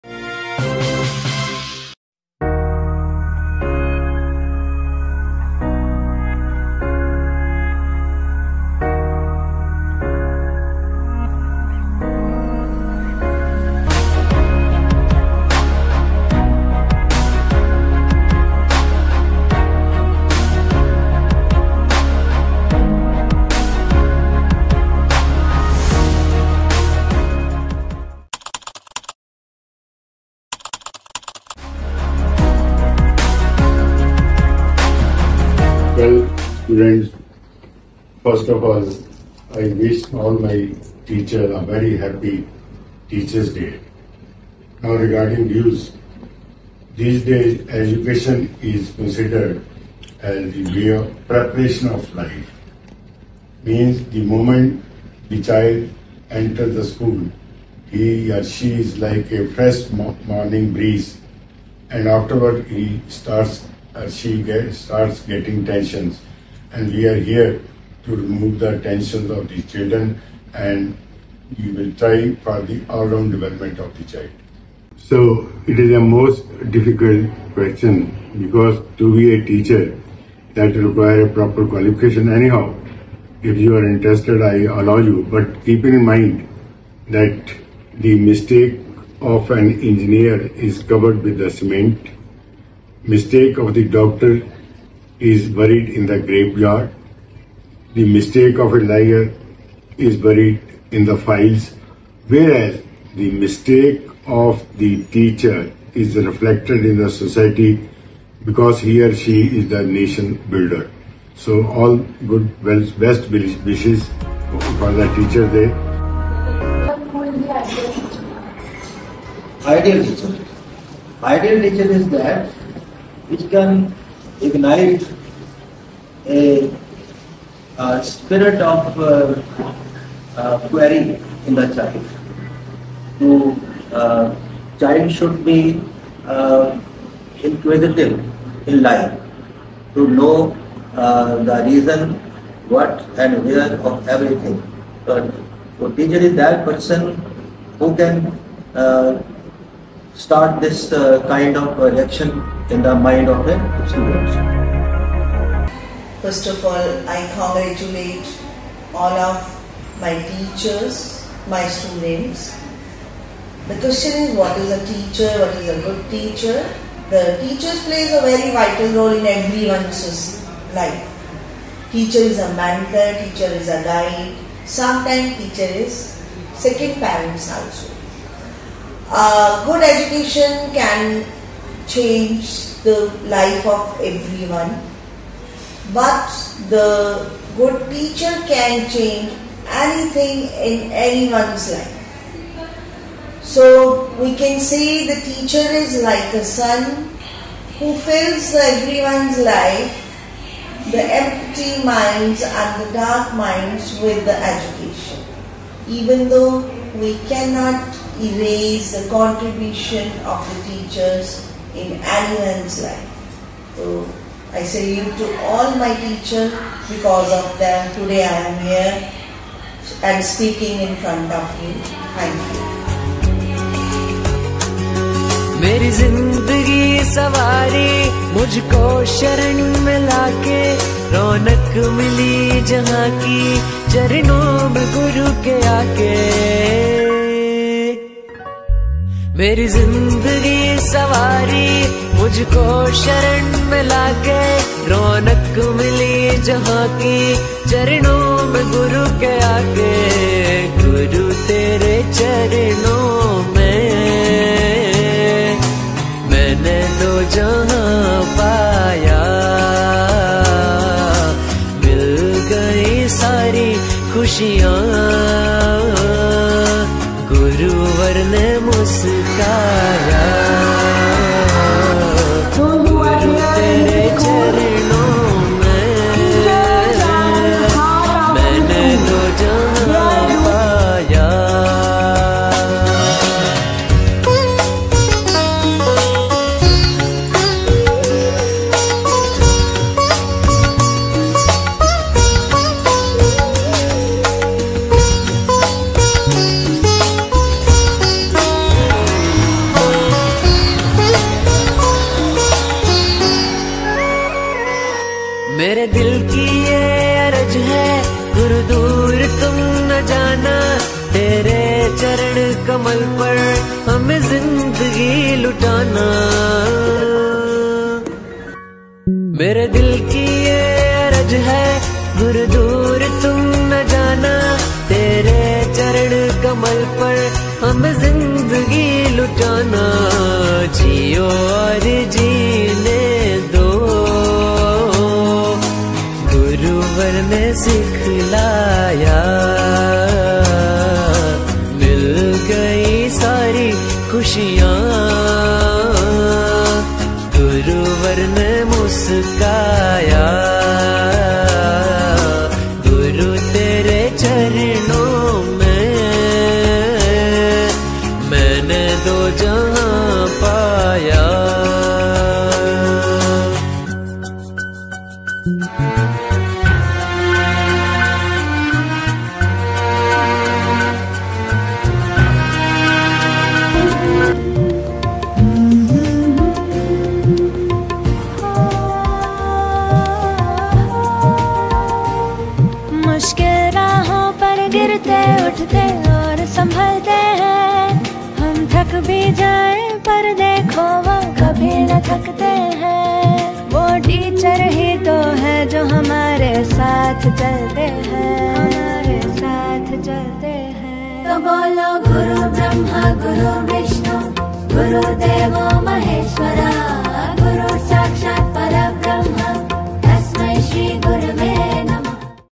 A sneak peek into Teachers Day celebrations held at SSDPS, Naya Nangal
Available on